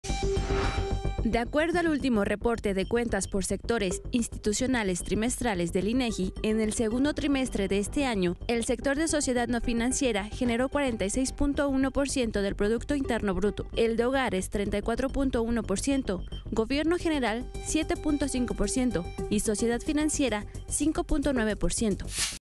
Sector de sociedad no financiera generó 46.1% del PIB en el segundo trimestre de 2023 | Paco Zea
De acuerdo al último reporte de cuentas por sectores institucionales trimestrales del INEGI, en el segundo trimestre de este año el sector de sociedad no financiera generó 46.1% del PIB.